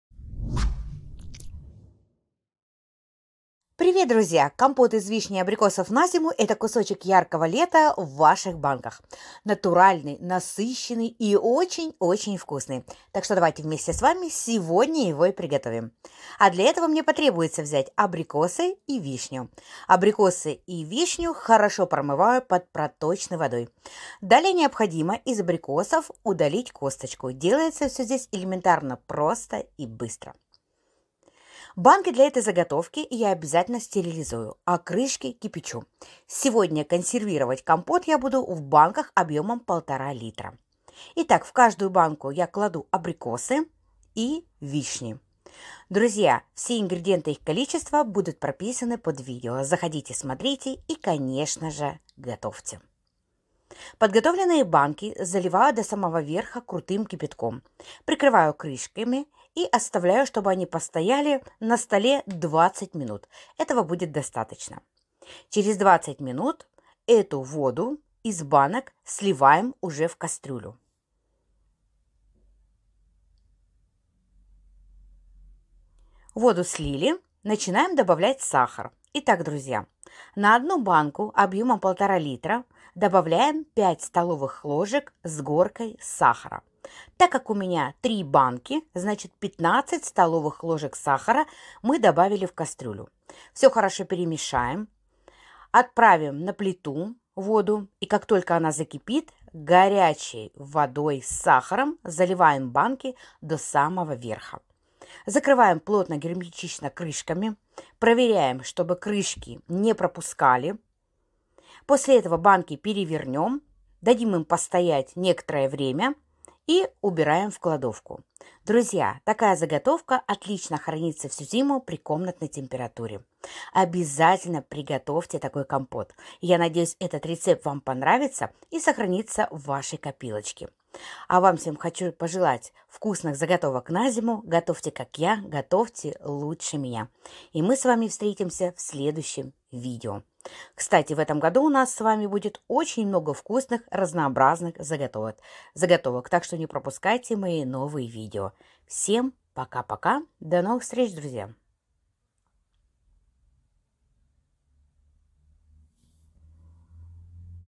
🔥КОМПОТ "АССОРТИ" на ЗИМУ 🍏🍒☘🍋🥤ЗАЛИВАЕМ и ЗАКАТЫВАЕМ!
нам потребуется:
абрикосы в банку 8 шт
вишня 25 шт
сахар 5 ст.л с горкой на 1 банку 1.5л
Приятных и вкусных Вам заготовок!
Спасибо Всем!